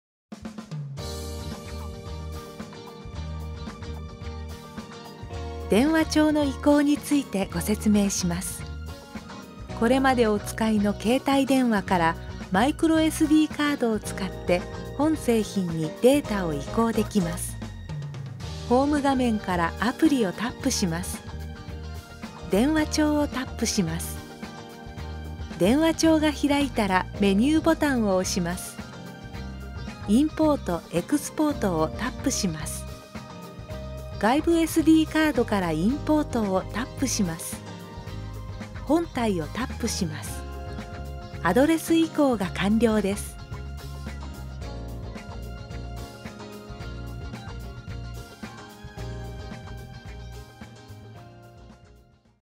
【GALAXY SIII Progre SCL21】電話帳・Eメールの移行
GALAXY SIII Progre SCL21で電話帳とEメールを移行する操作方法をご紹介します。

詳細はWEBサイトをご覧ください。